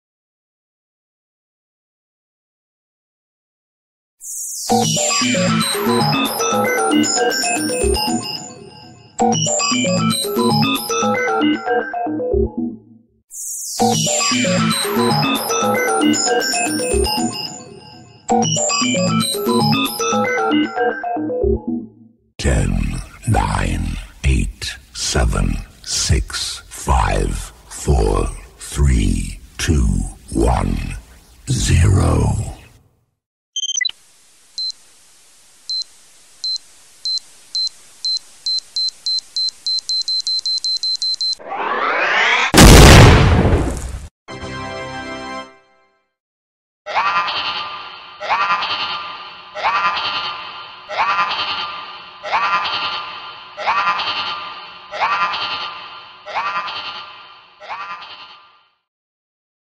☺ Lego Space Shuttle Explorer
Building of Space Shuttle brick by brick.